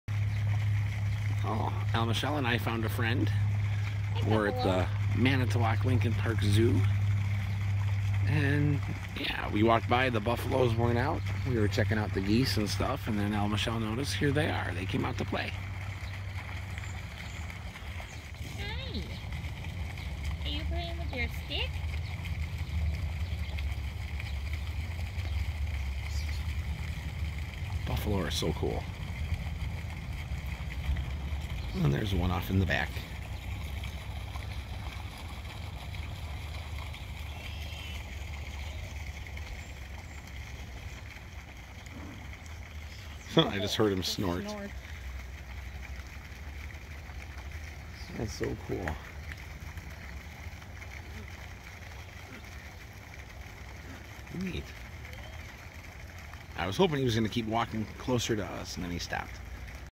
American Bison Manitowoc Lincoln Park Zoo Manitowoc, WI 6-25-22
L. Michelle and I stopped off at the free zoo to see what's around, and the bison finally decided to come out!